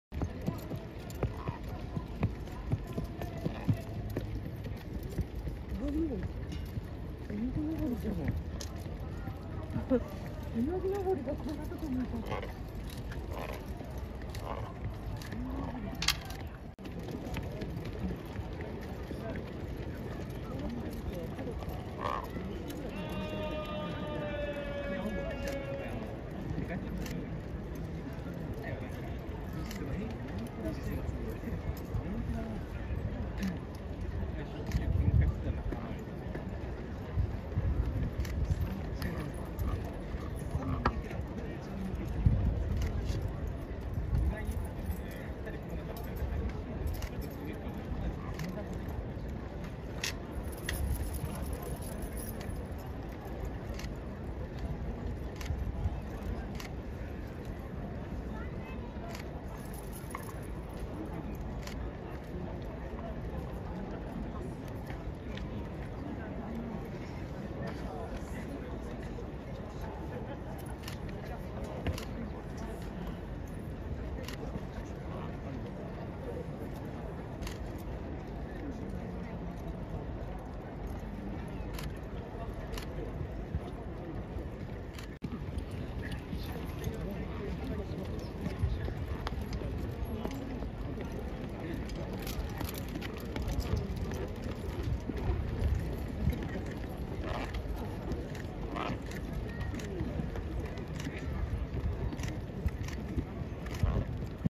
最近成績もうなぎ上りなウナギノボリ、勝負服柄のメンコもよくお似合い
低迷期から脱出してオープン入りも近いんじゃない？！😳

2023/4/23  東京競馬場　第10レース　鎌倉ステークス